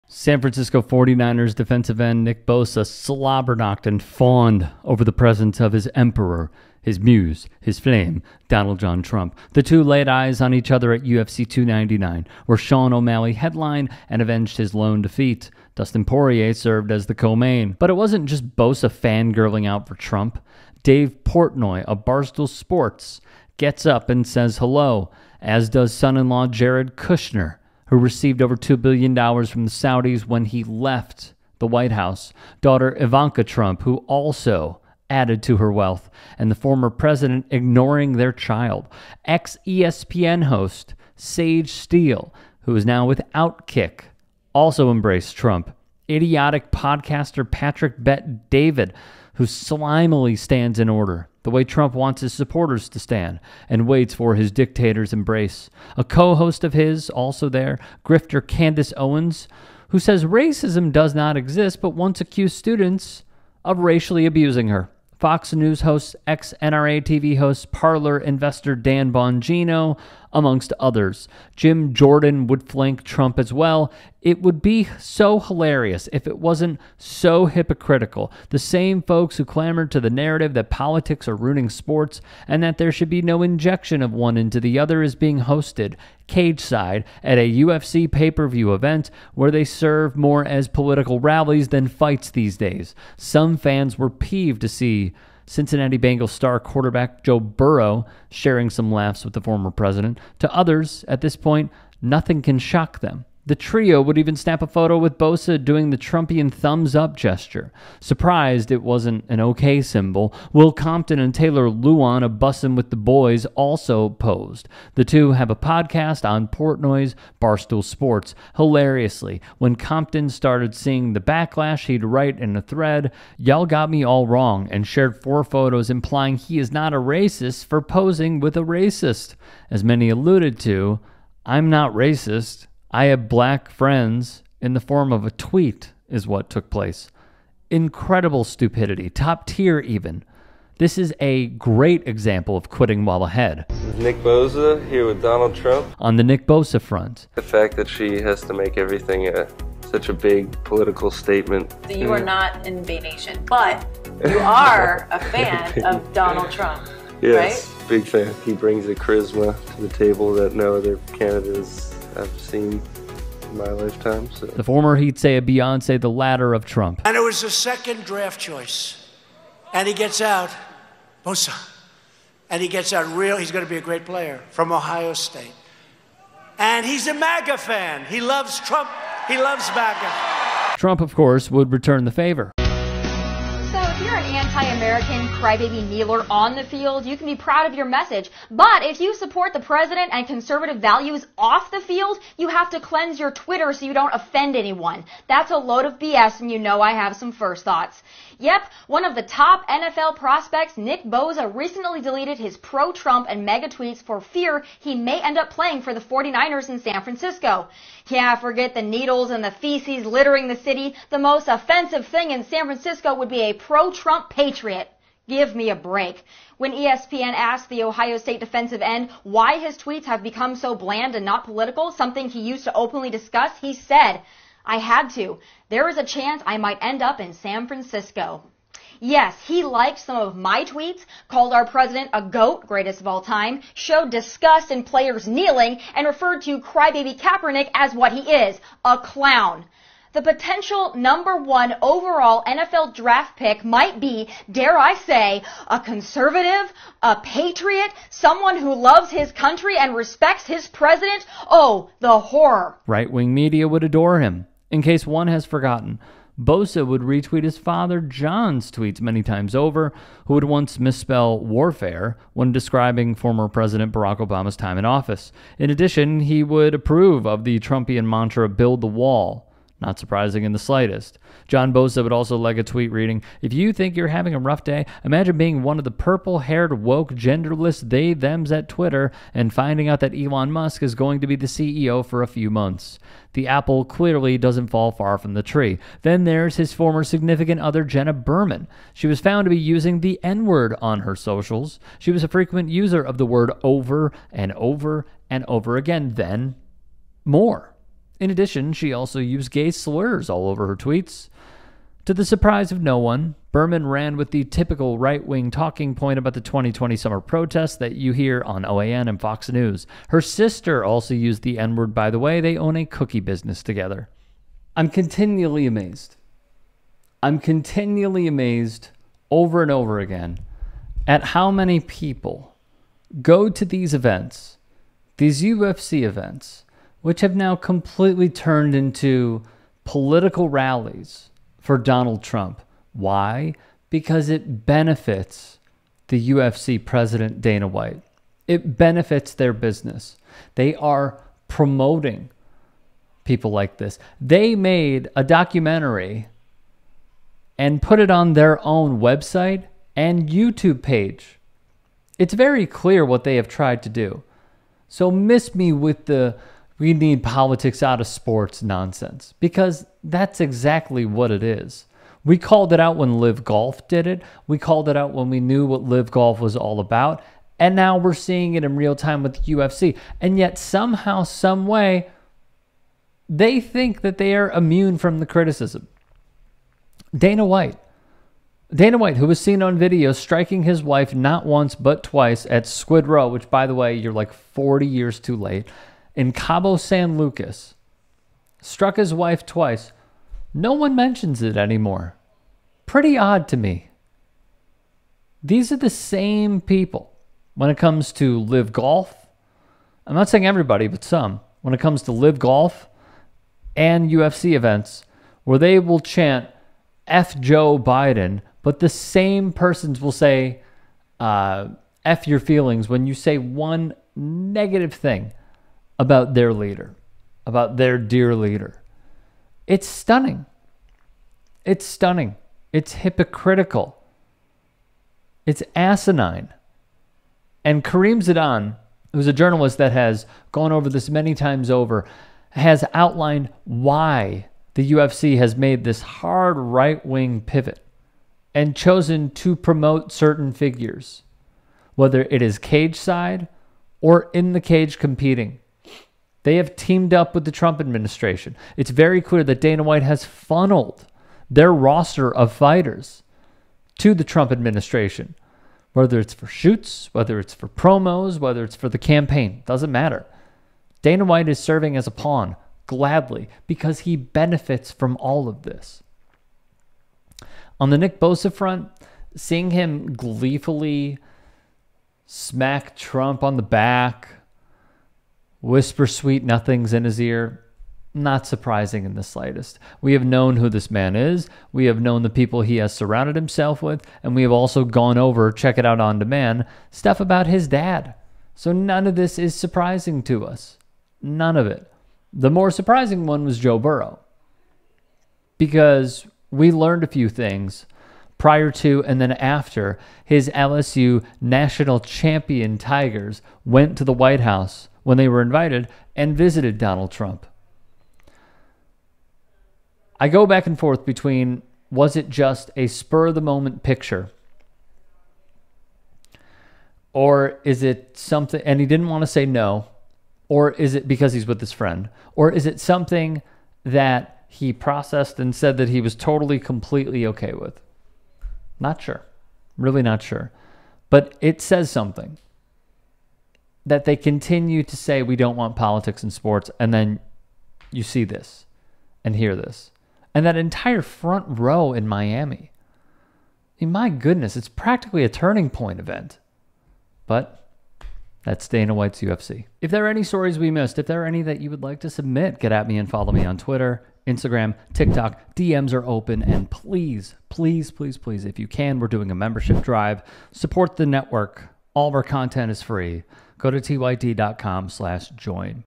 Trump Stooges SLOBBER Over Donald Trump at UFC 299
UFC 299 turned into a right-wing athlete and grifter convention as Donald Trump chummed it up with Nick Bosa, Joe Burrow, Dave Portnoy, Taylor Lewan, Will Compton, Patrick Bet-David, Sage Steele, Candace Owens, and more unsavory MAGA goons. Rick Strom breaks it down.

Rick Strom